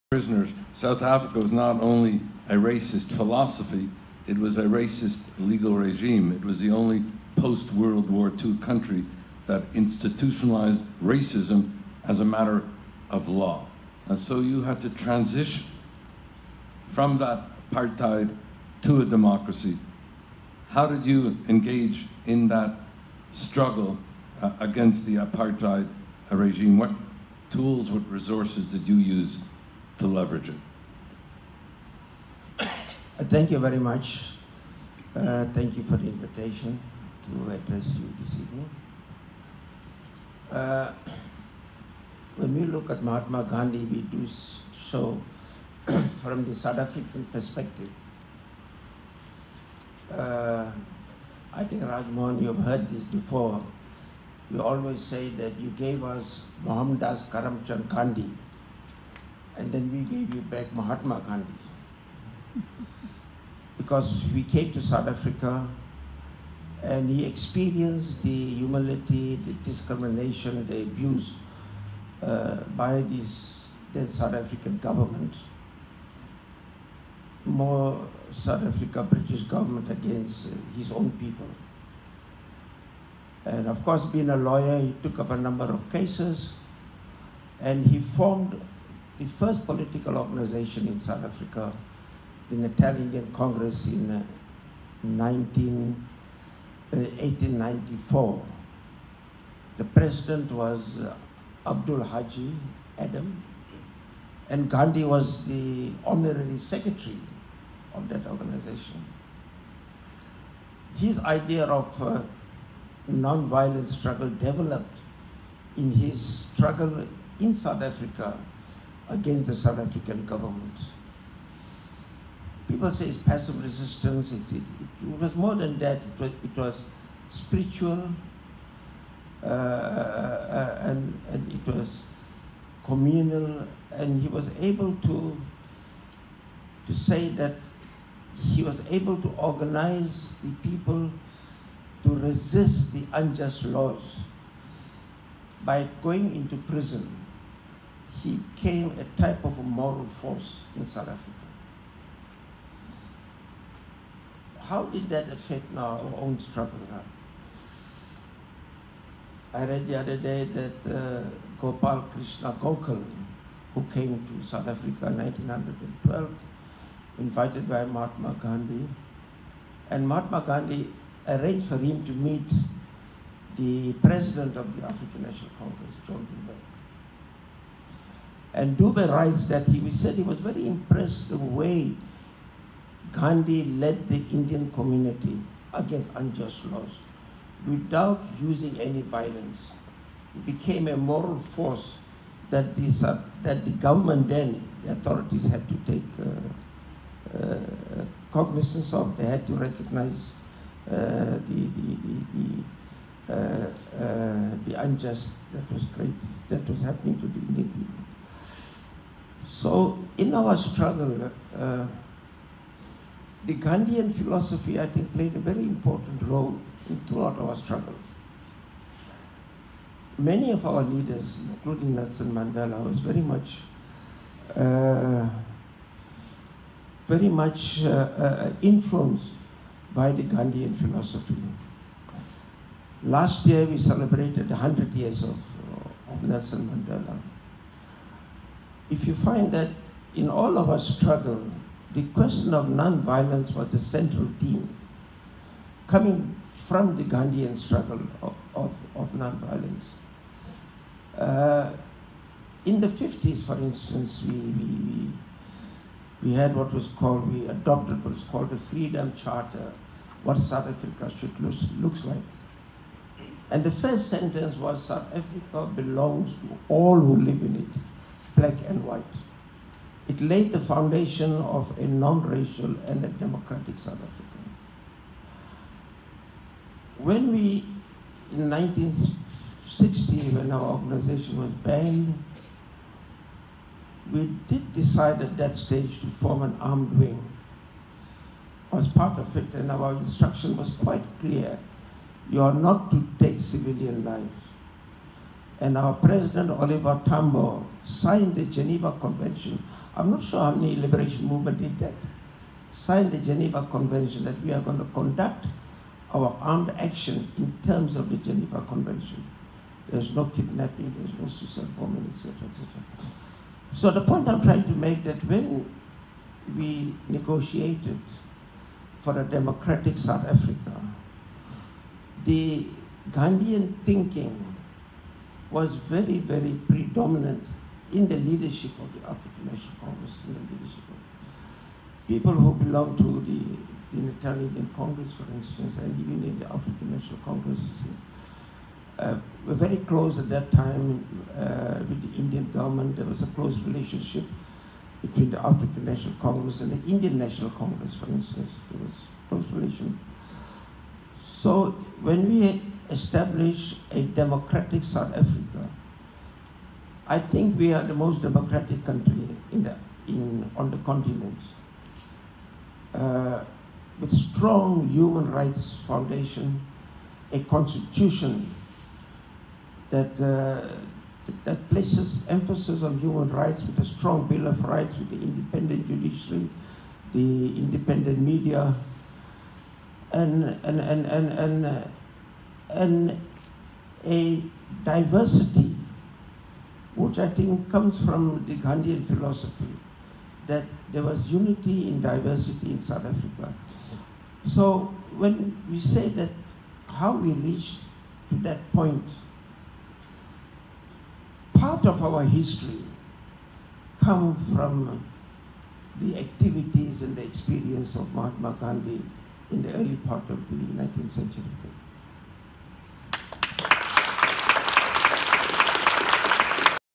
Ebrahim Ebrahim, Deputy Minister, African National Congress, South Africa
The Panel Discussion on "Mahatma Gandhi and the Renewal of Modern Democracy" held in Ottawa on May 13th, 2019 in partnership with University of Ottawa's Institute for Fiscal Studies and Democracy (IFSD).

This event was also part of this year's activities to mark the 150th anniversary.